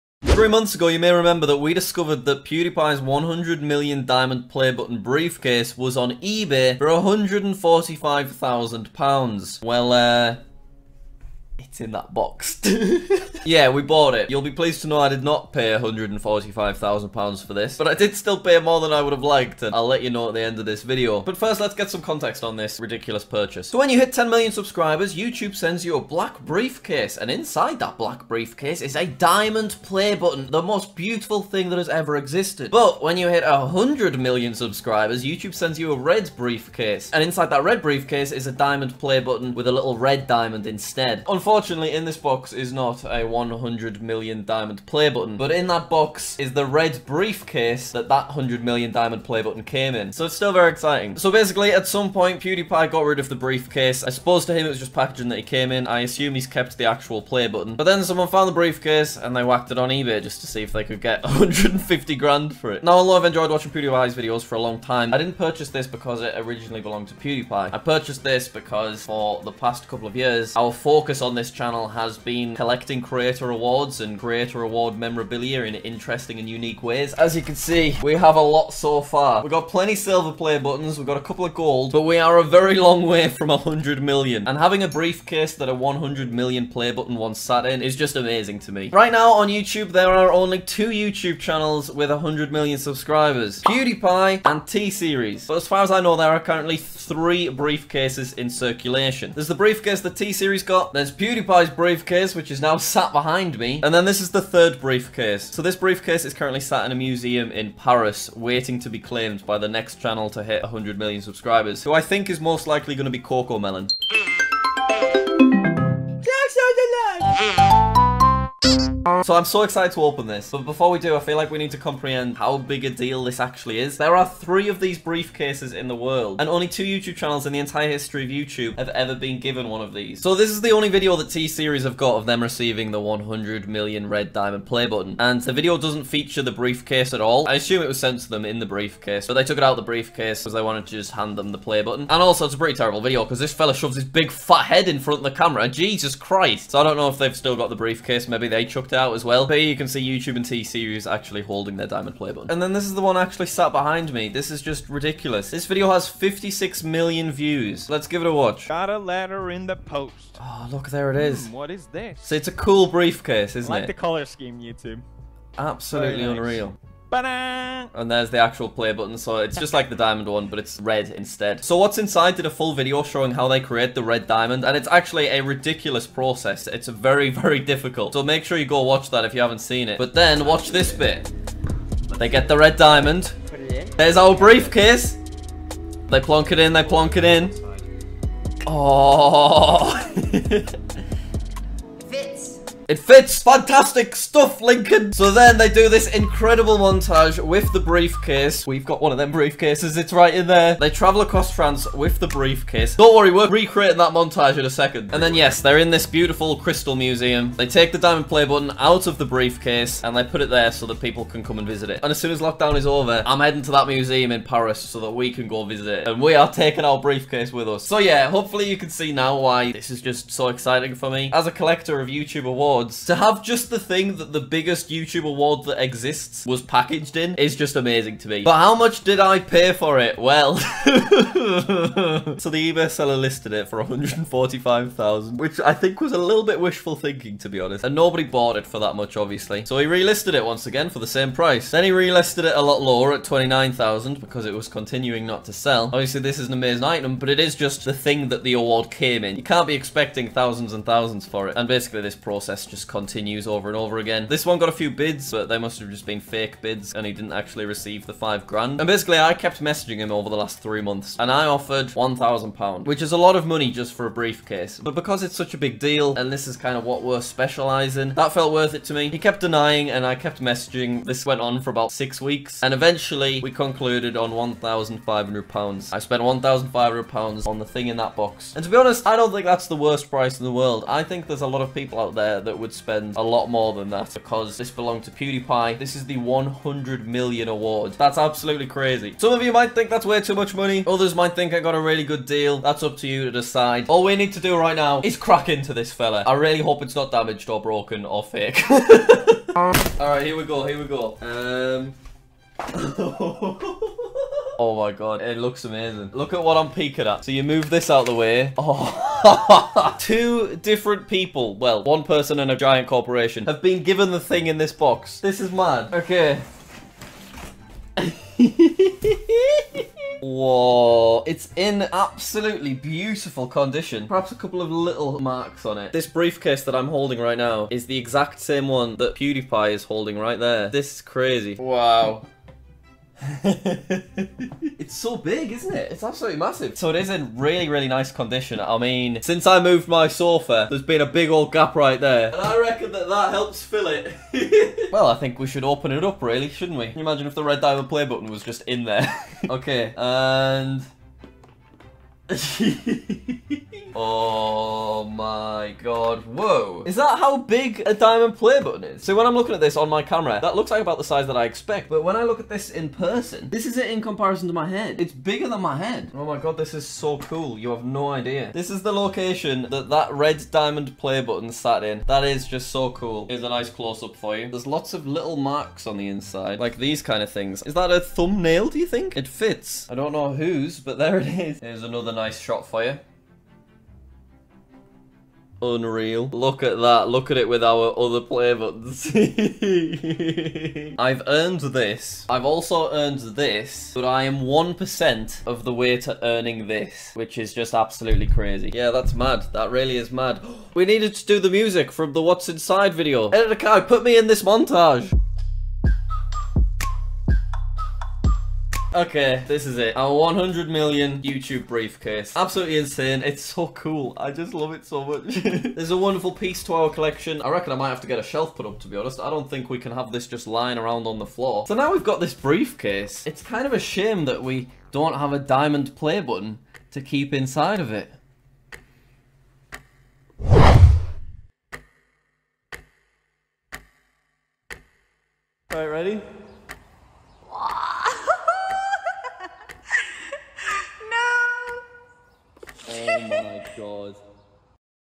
I found Pewdiepie's 100 Mill Diamond Play Button Chest on eBay (CDs Version) (by JackSucksAtLife)
After searching for play buttons, Jack found PewDiePie's 100 Million Diamond Play Button Chest on eBay.